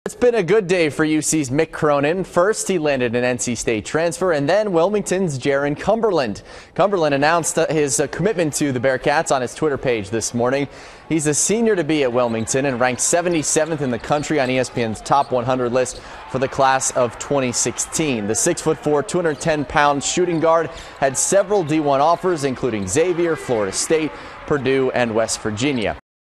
Cumberland Commits to UC
WILMINGTON -- Wilmington senior-to-be Jarron Cumberland committed to Cincinnati, he announced on his Twitter page Wednesday. Cumberland's ranked No. 77 on ESPN's Top 100 list for the class of 2016. The 6'4", 210-pound shooting guard had several DI offers, including Xavier, Florida State, Purdue and West Virginia. It was part of a very good day for UC's Mick Cronin. The Bearcats also landed N.C. State transfer Kyle Washington.